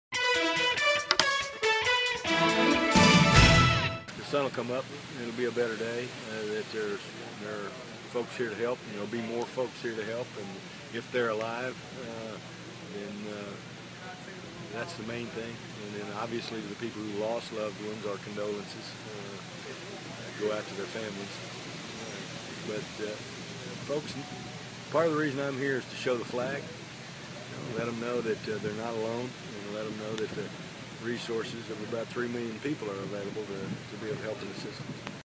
WEB EXTRA: Gov. Beebe Visits Denning
Arkansas Gov. Mike Beebe visits tornado-ravaged Denning, Ark.